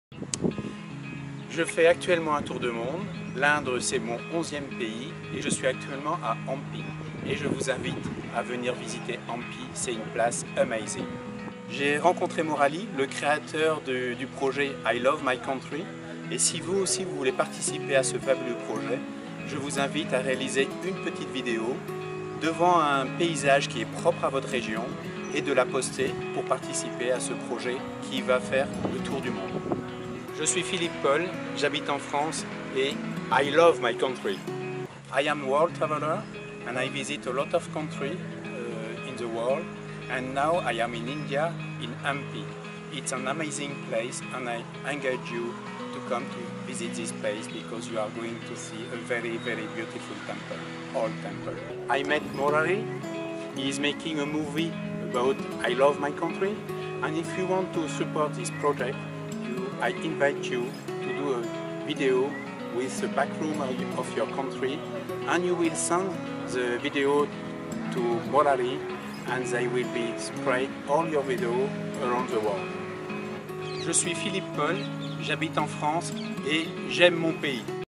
Philippe Paul from France inviting everyone to | Hampi | I Love My Country The Film
Hey friends, Hampi is calling you here today our 256 ilovemycountrythefilm post features is "Philippe Paul from france" thank you for your great love and support for our ilovemycountrythefilm project and being a pat of it.

With big love:
Written and Directed by
Muralidhar Gidijala and All our team members.